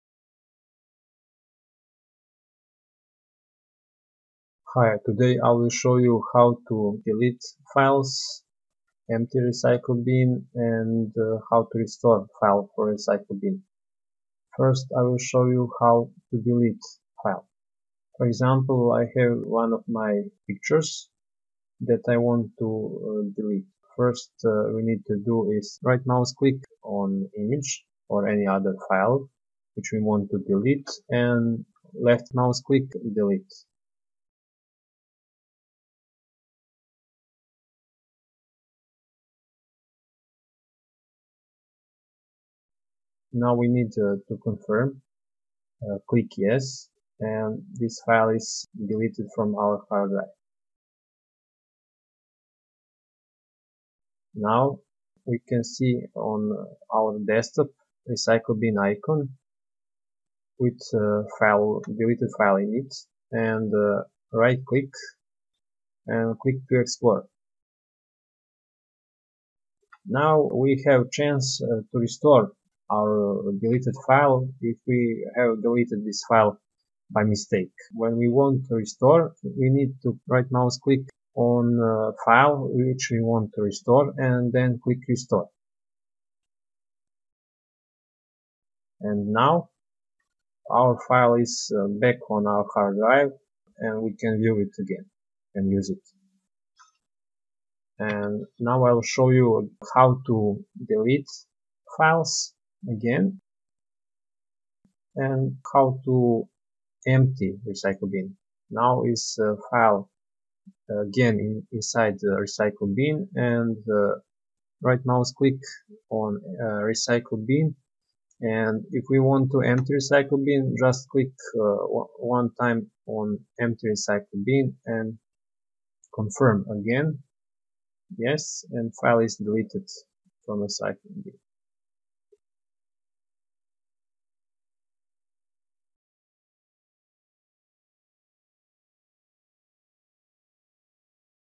How to delete files restore files and empty recycle bin windows xp
This is tutorial made for beginners how to on windows xp system easily to delete files, restore file and empty recycle bin simple explained tutorial in few steps.